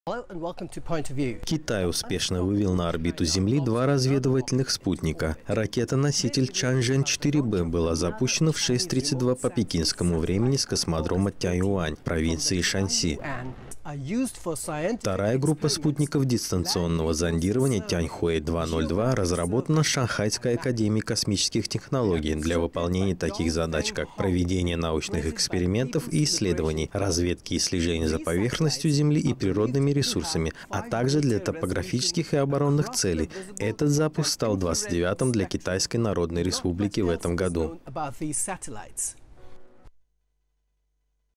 Китай вывел на орбиту Земли два разведывательных спутника
Китай успешно вывел на орбиту Земли два разведывательных спутника. Ракета-носитель "Чанчжэн-4В" была запущена в 6:32 по пекинскому времени с космодрома Тайюань в провинции Шаньси.

Вторая группа спутников дистанционного зондирования "Тяньхуэй II-02"  разработана Шанхайской Академией космических технологий для выполнения таких задач, как проведение научных экспериментов и исследований, разведки и слежения за поверхностью земли и природными ресурсами, а также для топографических и оборонных целей. Этот запуск стал 29-м для Китайской Народной Республики в этом году.